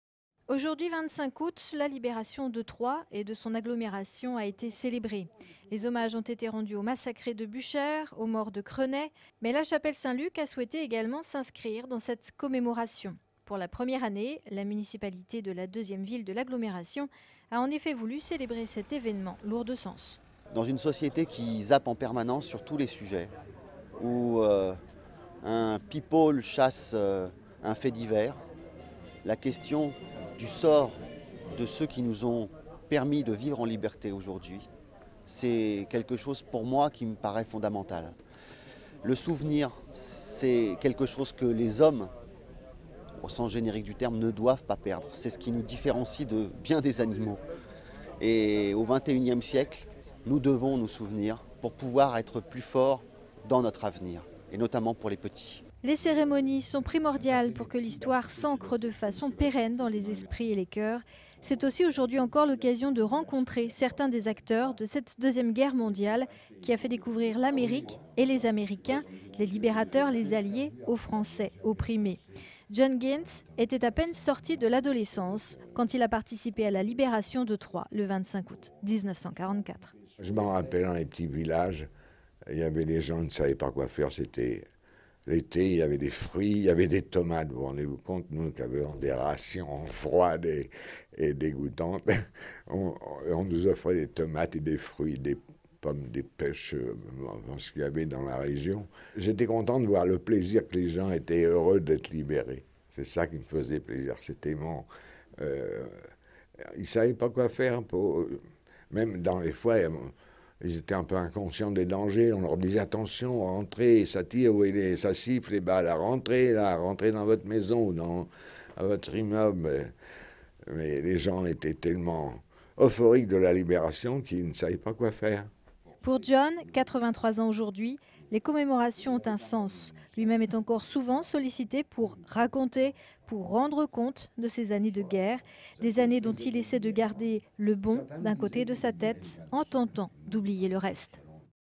Commémoration de la libération de Troyes
Focus sur la 66ème commémoration de la libération de Troyes et de son agglomération. (DROITS RESERVES . Pour toute exploitation sur TV, web, mobile, aller